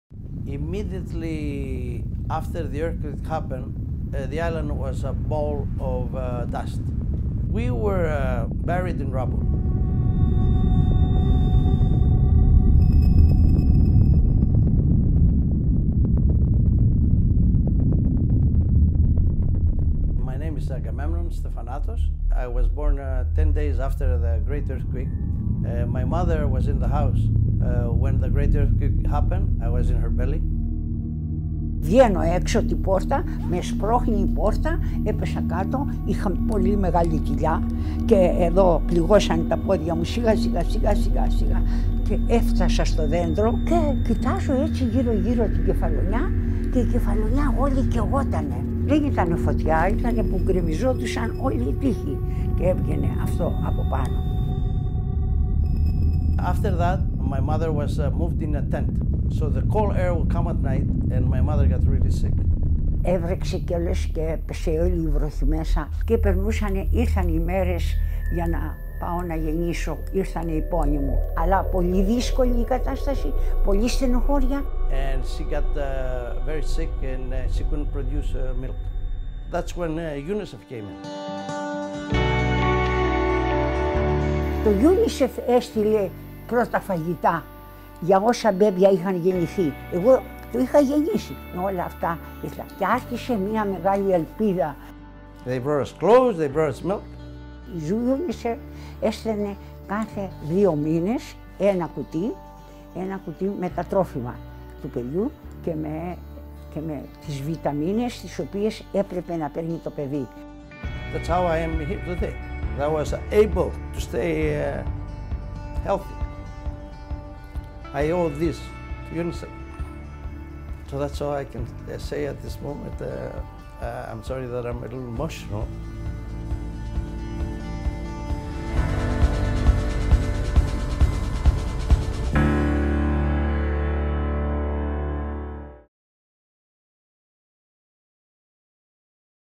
Dagur rauða nefsins 2017 - UNICEF í 70 ár
UNICEF hefur í yfir 70 ár barist fyrir réttindum barna út um allan heim og veitt börnum í neyð lífsnauðsynlega hjálp.

Hér heyrum við sögu Agamemnon Stefanatos. Hann og móðir hans fengu aðstoð frá UNICEF eftir að mikill jarðskjálfti reið yfir eyjuna Cephalonia í Grikkland fyrir rúmum 60 árum. Hann var þá nýfæddur.

Innslagið var sýnt á degi rauða nefsins árið 2017.

Dagur rauða nefsins er langstærsti viðburður ársins hjá UNICEF á Íslandi. Með honum vill UNICEF skemmta fólki og vekja um leið athygli á að heimsforeldrar og UNICEF berjast saman fyrir betri heimi fyrir öll börn.